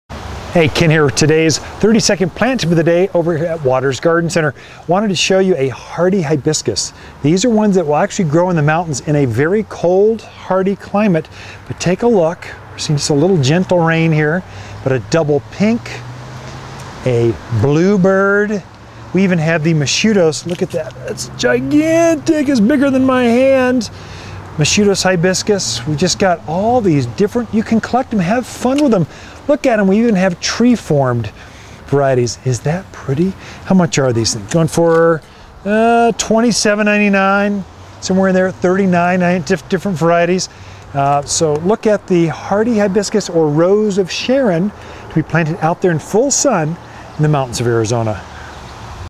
Rose of Sharon in Bloom.MPG
Rose Of Sharon, Hibiscus syriacus - Eye-catching, summer-flowering shrub, selected for longer blooming season, exceptional flowers, little to no seed production and abundant continuous flowering.  Handsome, leathery, dark green leaves and a dense, compact growth habit. Highly tolerant to air pollution and drought. Makes a wonderful informal hedge or screen.